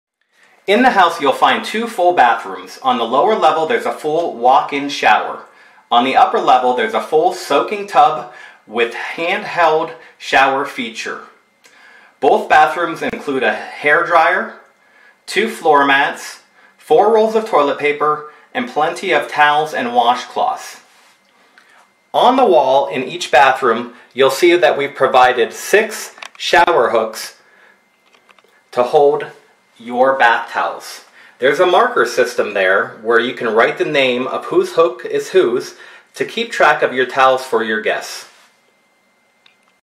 Bathroom amenities
Tutorial on features of our bathrooms & towels.